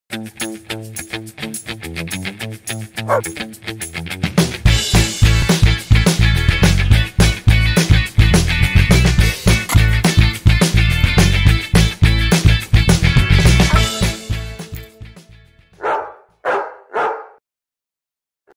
Bazooka na tenisové míčky pro psy | Krmiva u Toma
►►► Zabavit vašeho čtyřnohého kamaráda lze různými způsoby. Jedním z nich může být střelba tenisových míčků.

►►► Máte dotazy ohledně našeho sortimentu? Máte nápad na zlepšení? Líbilo se vám naše video? Napište nám to dolů do komentářů!